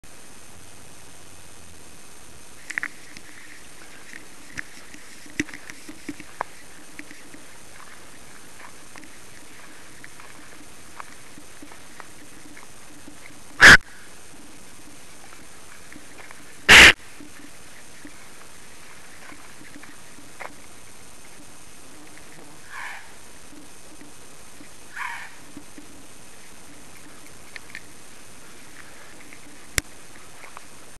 Gray Fox & Call
I captured footage of a gray fox in our neighborhood and its call. I am balancing a powerful flashlight and my German shepherd Magnum while recording the film. There are twenty-seven or so sub-species of gray fox. This has a black-tipped tail and is about two feet in length not counting the tail. Eyes appear bright yellow in the flashlight beam.
At the start, it is me calling to the fox, later you will hear it call to me.